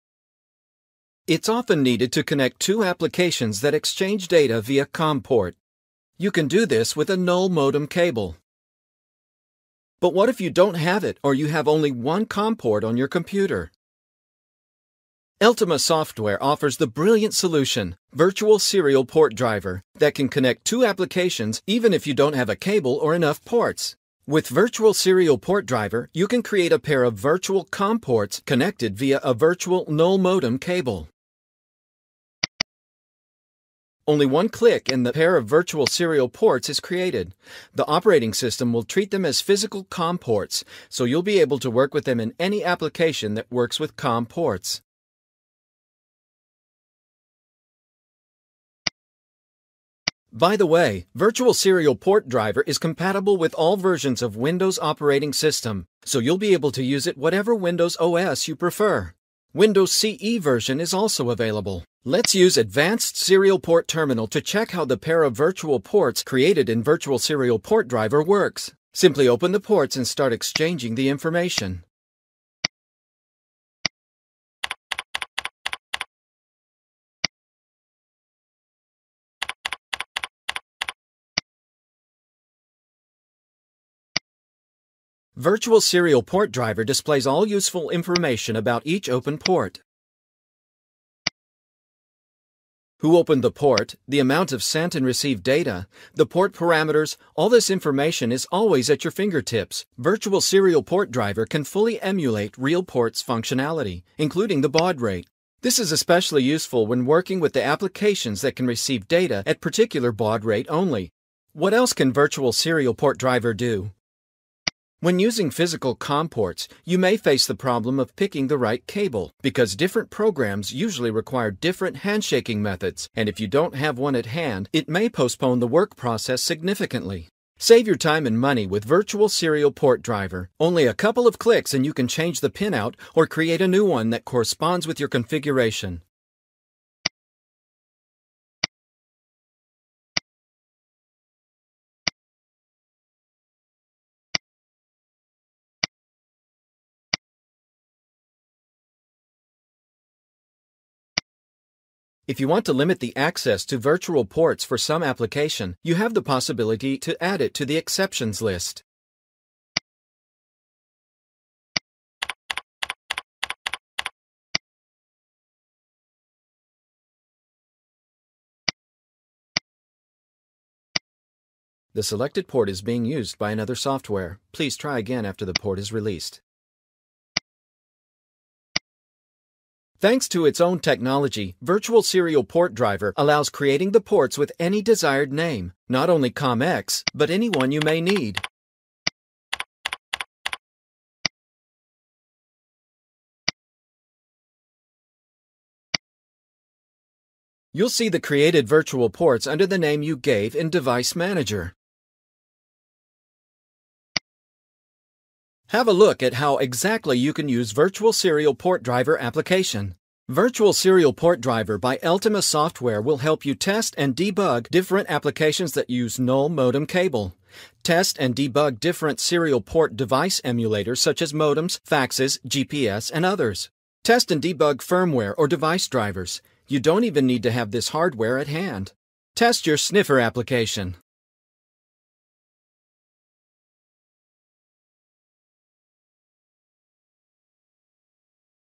How to create virtual serial port
Need a virtual serial port on your PC to test and debug your applications? Here is how you do it.